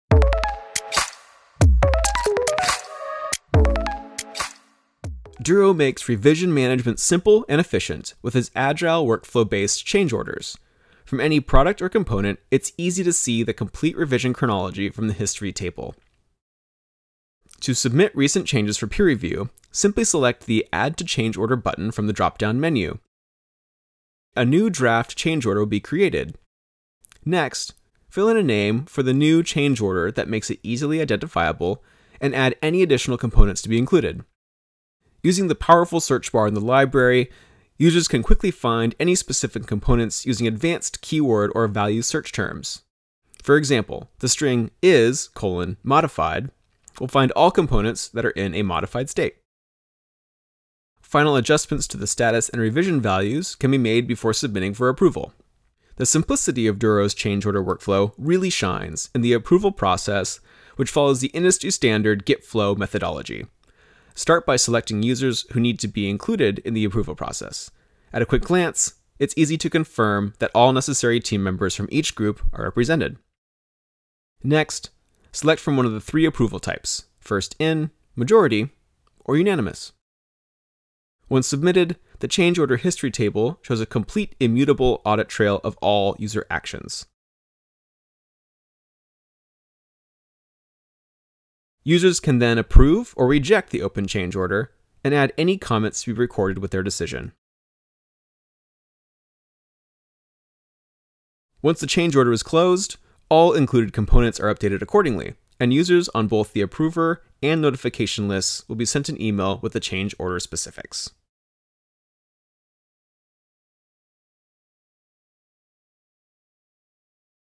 Creating and Submitting a Change Order
Duro makes revision management simple and efficient with it’s agile workflow based Change Orders. From any Product or Component it’s easy to see the complete revision chronology from the History table.

To submit recent changes for peer review, simply select the Add To Change Order button from the drop down menu

The simplicity of Duro’s Change Order workflow really shines in the Approval process which follows the industry standard git-flow methodology. Start by selecting users who need to be included in the approval process. At a quick glance, it’s easy to confirm that all necessary team members from each Group are represented.

Once submitted, the Change Order History table shows a complete immutable audit trail of all User actions.

Users can then Approve or Reject the open Change Order and add any comments to be recorded with their decision.